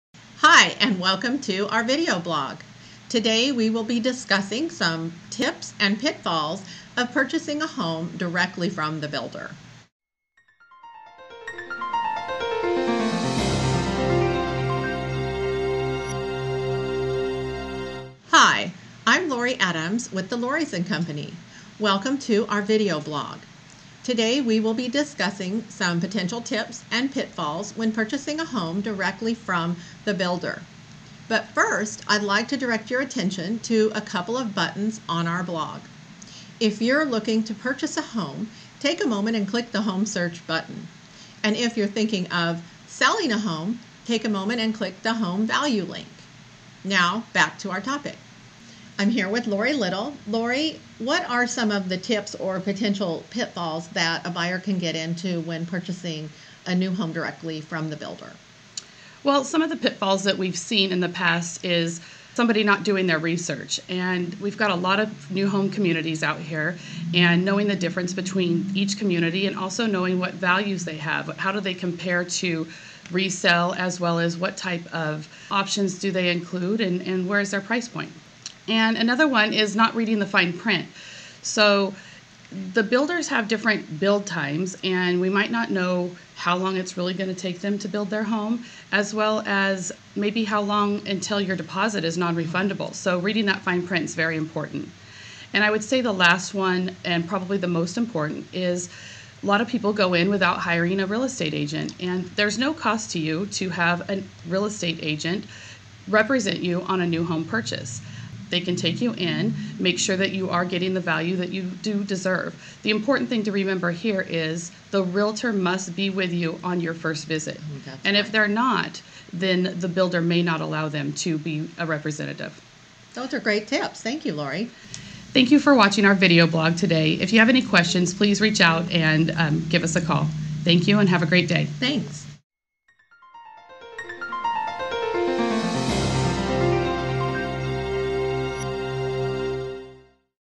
Central Valley Real Estate Team Tips for buying new construction
"There are three common pitfalls to avoid when buying a home directly from the builder. For example, some people don't hire a real estate agent. A real estate agent fights for your best interests. To learn more, watch this short video!

Buying a Central Valley Area Home? Search All Homes For Sale

Selling Your Central Valley Area Home?

The Lori's and Company
Manteca, California